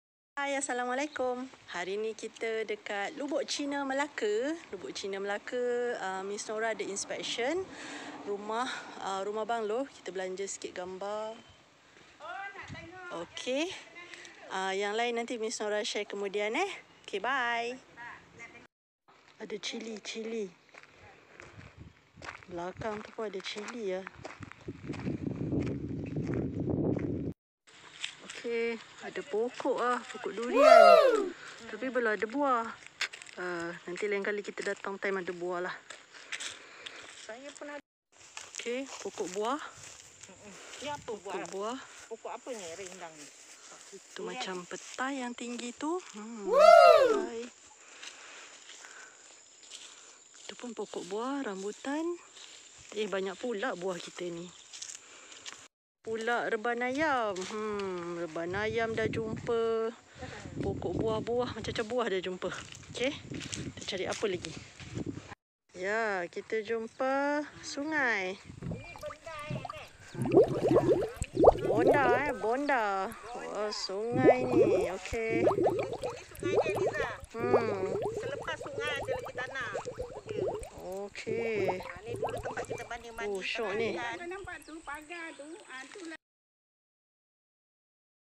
Inspection tanah dan rumah di Lubuk China Melaka.
Tanah mempunyai sebuah rumah anggaran keluasan 800sqf, 3bilik. Di belakang rumah ada tanah bekas sawah dan sungai.

Untuk gambar dan video sebenar boleh hubungi Ms Nora di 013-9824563.